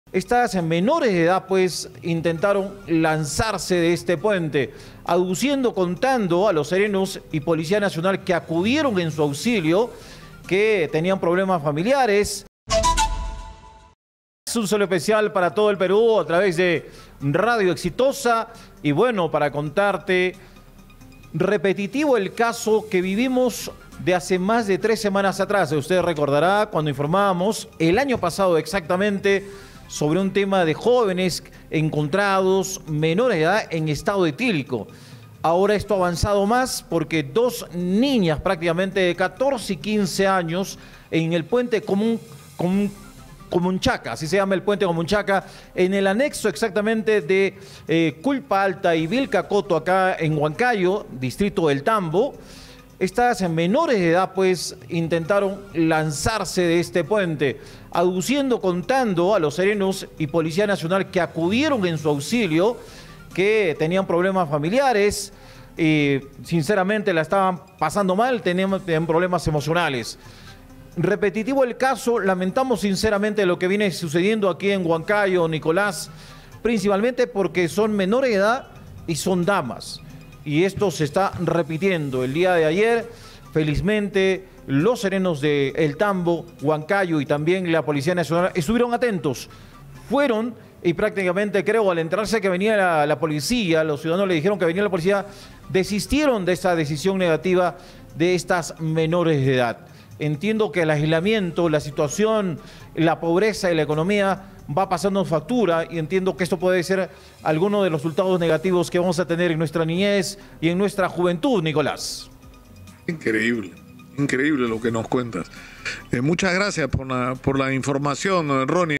🔴🔵Huancayo: Dos menores intentaron suicidarse al querer lanzarse del puente Comunchaca
Dos menores de edad intentaron suicidarse al querer lanzarse del puente Comunchaca, en el distrito de El Tambo, Huancayo, región de Junín.

Exitosa Perú
"La voz de los que no tienen voz"

Corporación Universal